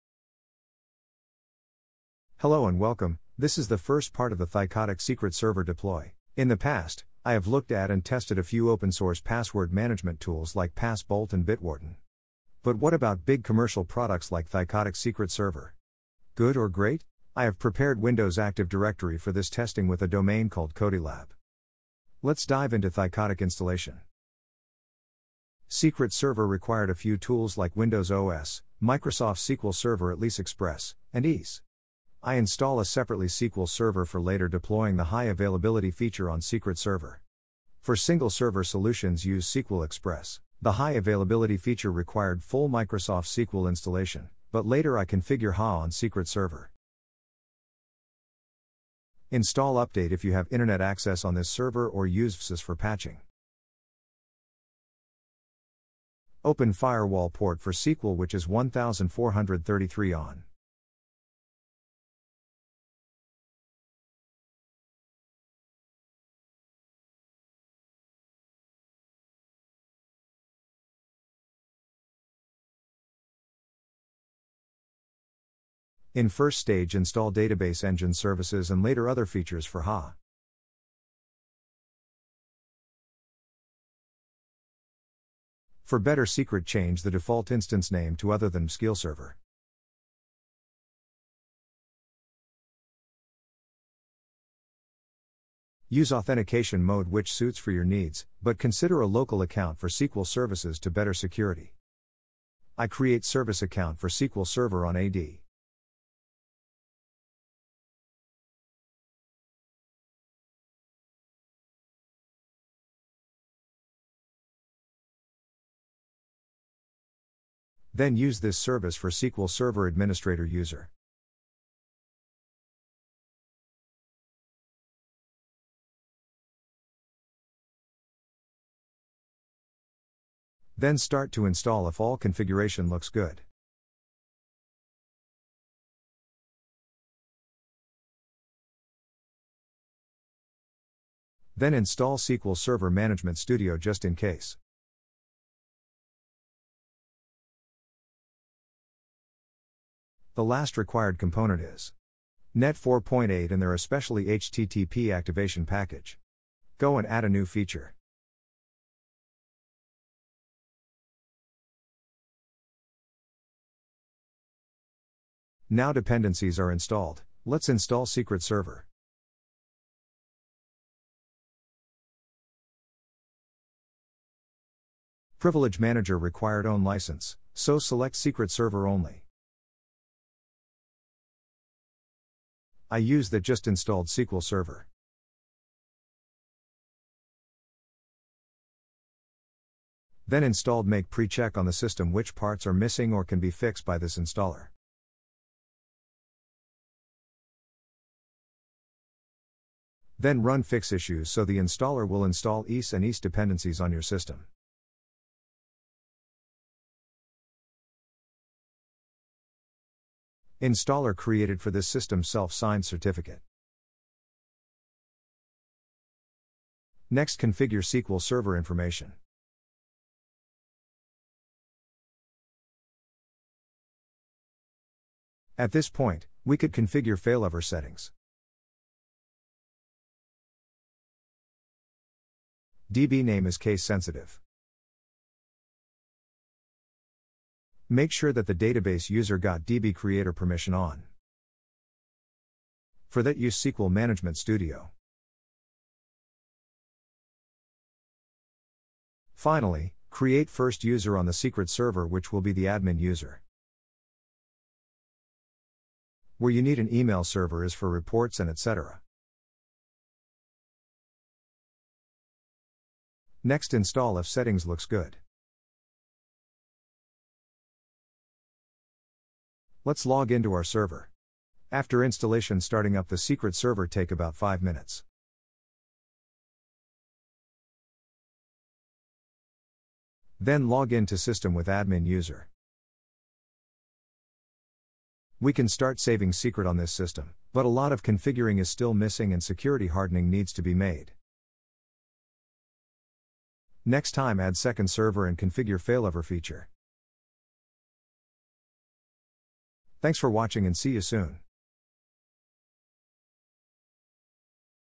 How to install Thycotic Secret Server
In the past, I have looked at and tested a few open-source Password management tools like pass bolt and bitwarden. But what about big commercial products like Thycotic Secret Server? Good or great?

There will be looking on failover feature and other feature like offline caching on mobile device